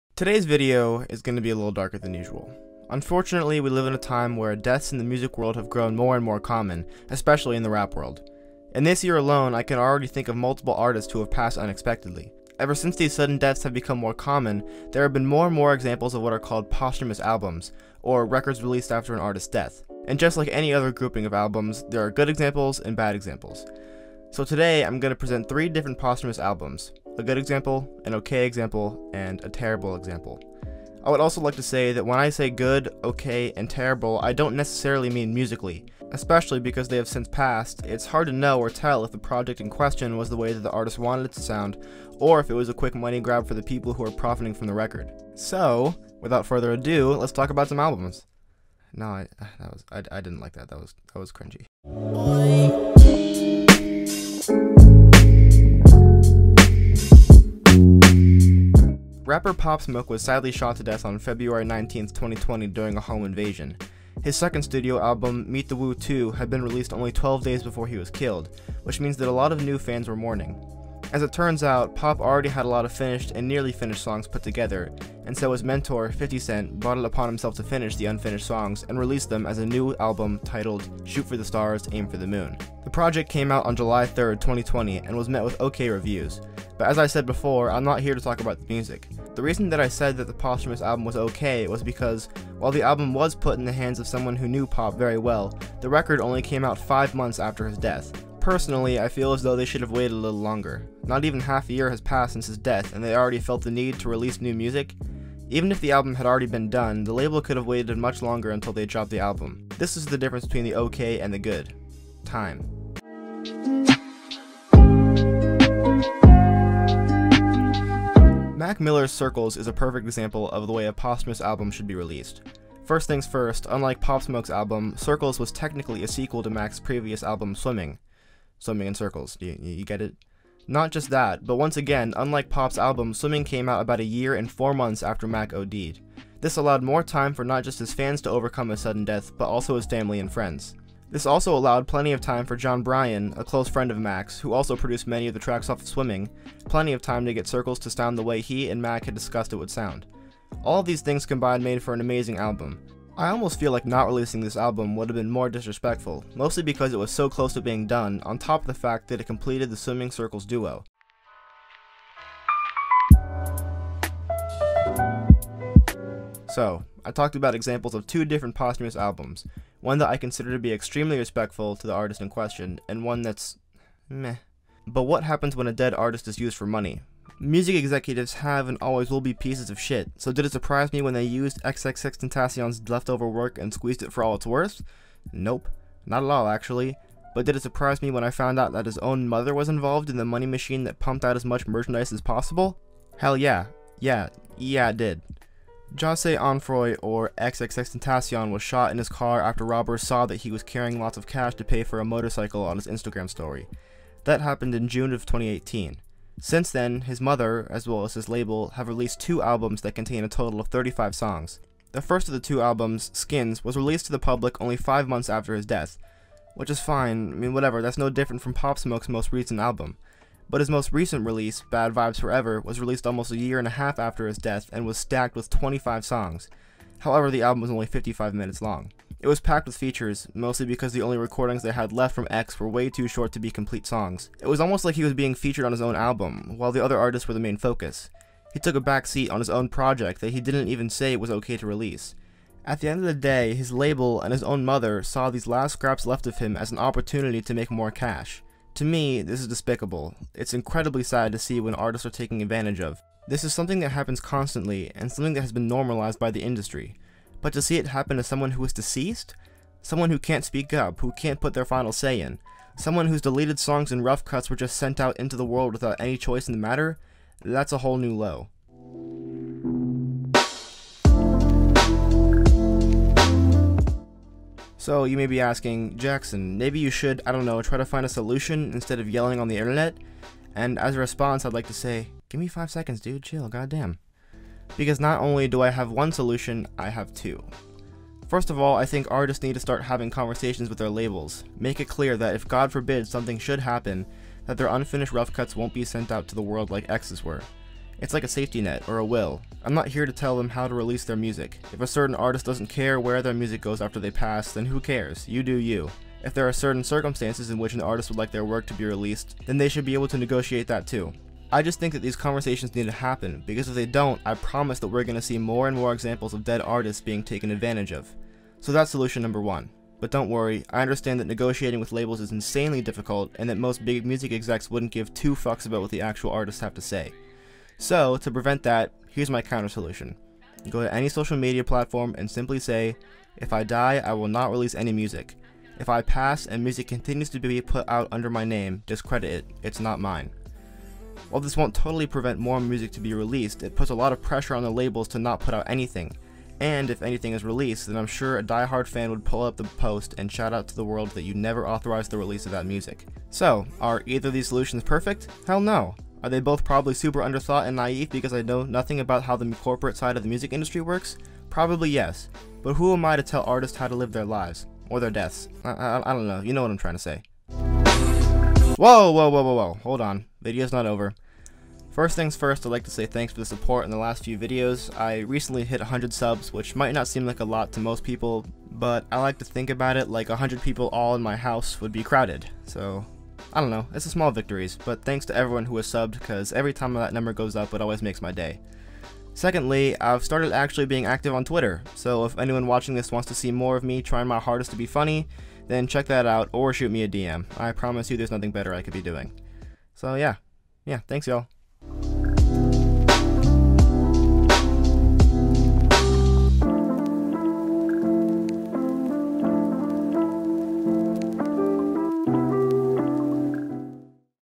POSTHUMOUS ALBUMS - The Do's and Don'ts
Over the last few years, releasing music after an artists has passed away has become more and more common. Is this a good thing or a bad thing? And what is the best way to release a posthumous album?